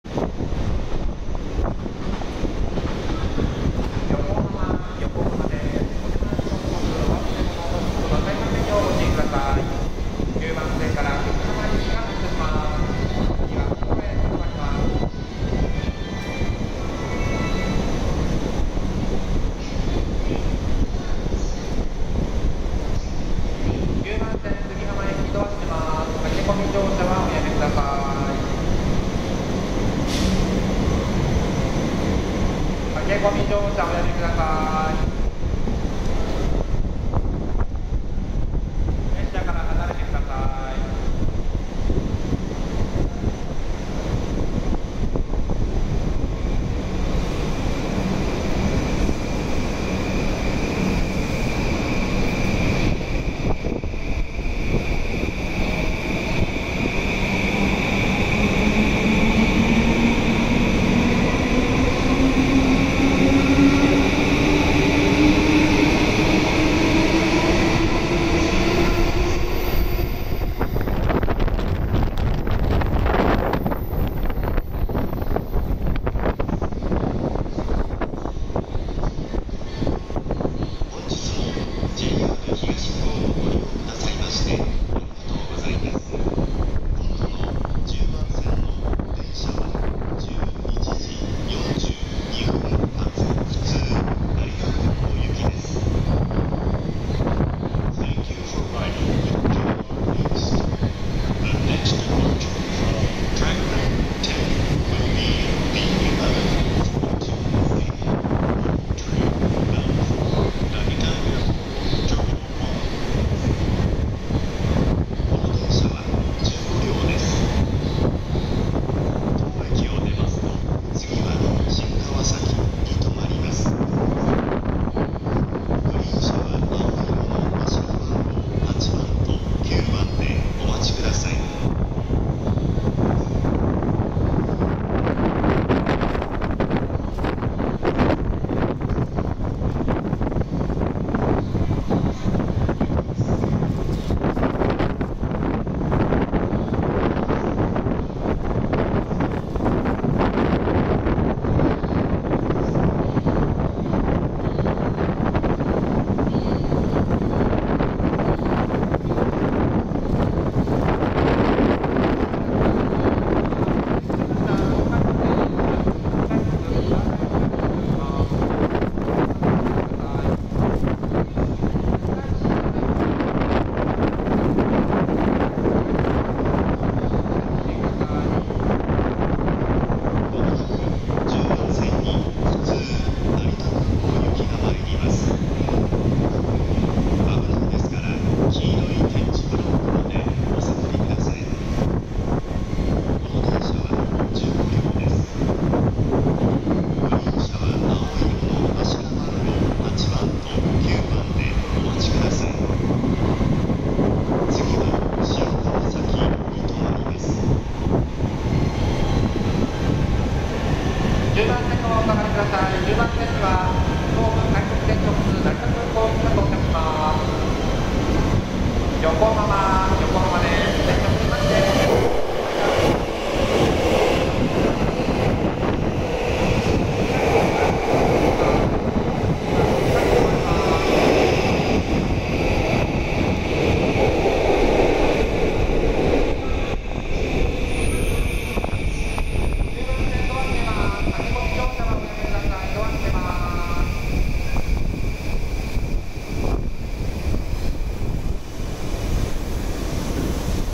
横浜駅に到着する横須賀線E235系電車5 Yokosuka line train E235 series arriving Yokohama Station 5
到着は3:33頃　ラーメン動画の素材に使用するために撮影しましたが風切音が酷くてボツ
横浜駅
Google map
〒220-0011 神奈川県横浜市西区高島２丁目